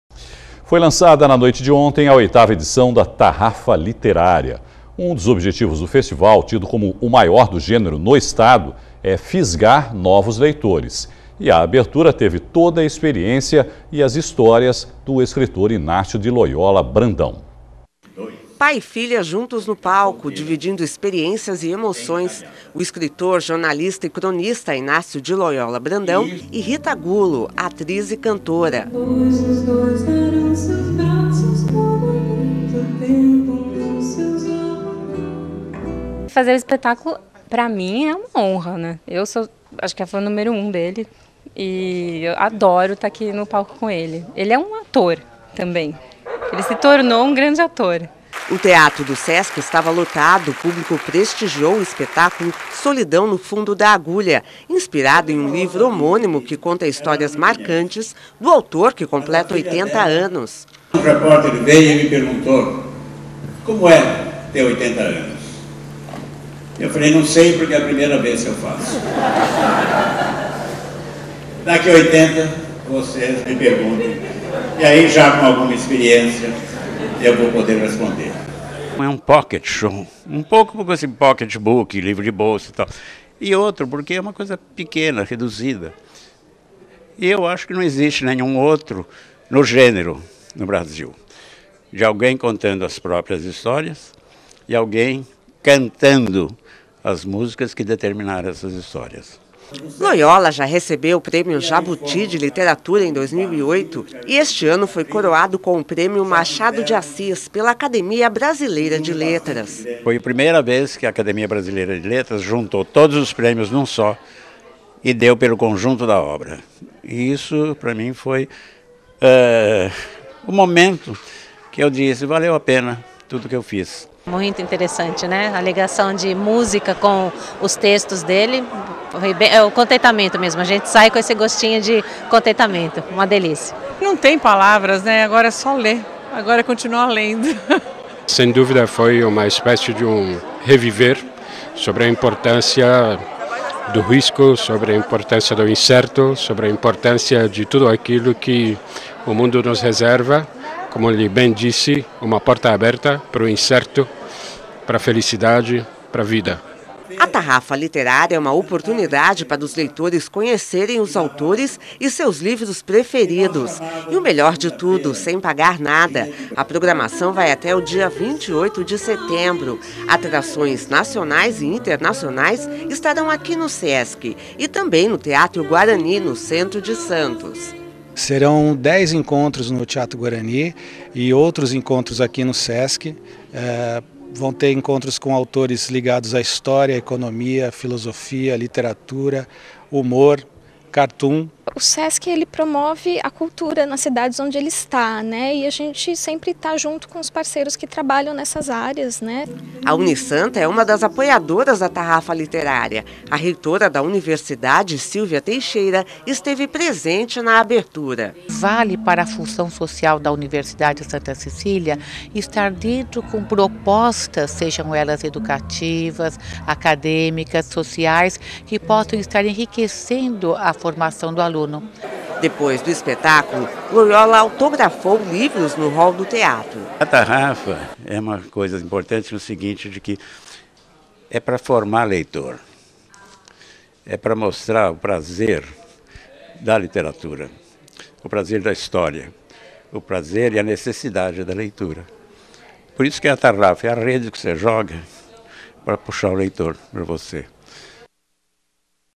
Festa de abertura da Tarrafa Literária contou com escritor Inácio de Loyola Brandão
A 8ª edição da Tarrafa Literária, maior festival literário do Estado de São Paulo, começou quarta-feira (21). A abertura coube ao escritor Inácio de Loyola Brandão, que dividiu o palco com a filha, a atriz e cantora Rita Gullo.